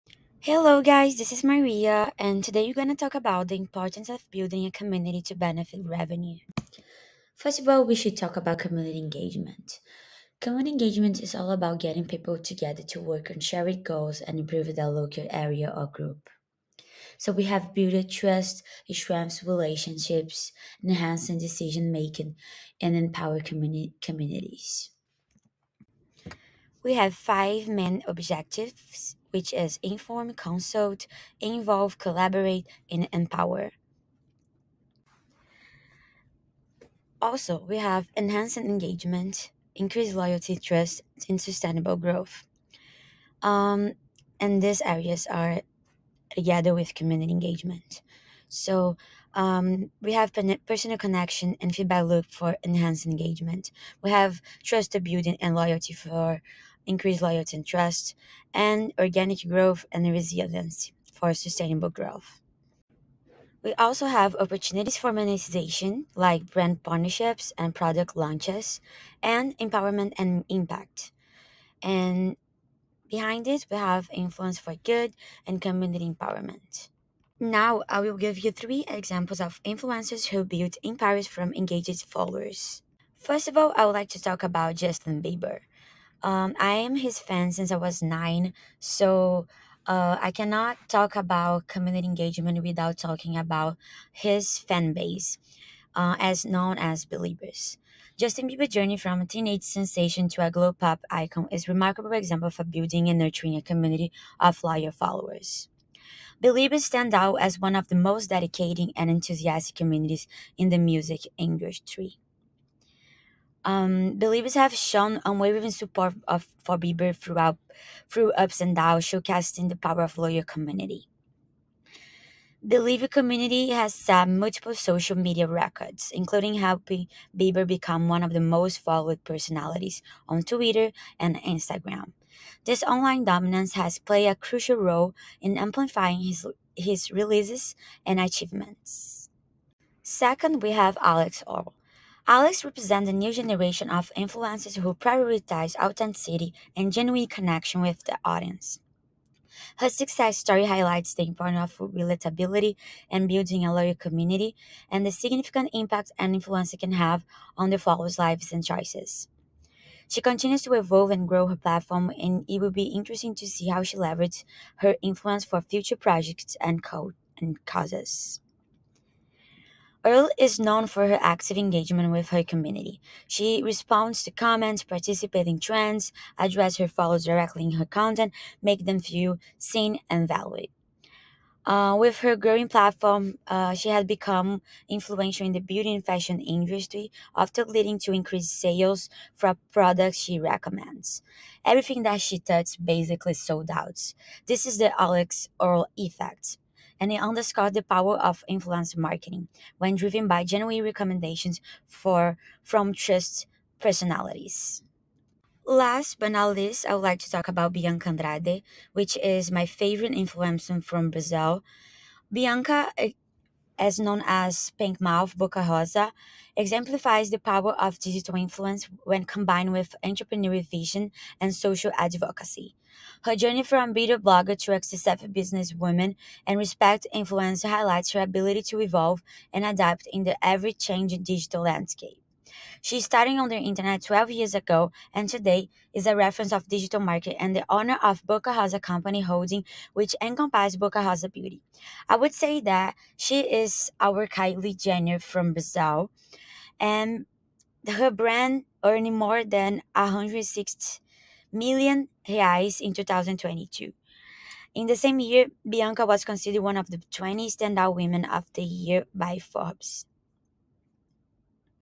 THE IMPORTANCE OF BUILDING A COMMUNITY TO BENEFIT REVENUE - MARKETING CONCEPT (Audio voice)
Creating a strong community around your brand is a powerful marketing strategy that can significantly enhance revenue growth. In this video, we delve into the importance of building a community in the context of marketing and how it can serve as a key driver for revenue enhancement. We'll explore the psychological underpinnings that make community building so effective, including the sense of belonging it fosters among consumers and the trust it builds in your brand. Additionally, we'll discuss practical strategies for cultivating a vibrant, engaged community across various platforms, from social media to your own website. Through real-world examples and expert insights, we'll demonstrate how successful brands leverage their communities to drive loyalty, enhance customer lifetime value, and ultimately boost their bottom lines. Whether you're a startup looking to establish a foothold in the market or an established company seeking to invigorate your brand, understanding the critical role of community in marketing is essential for achieving sustainable revenue growth. Join us as we unlock the secrets to harnessing the power of community to benefit your business's revenue.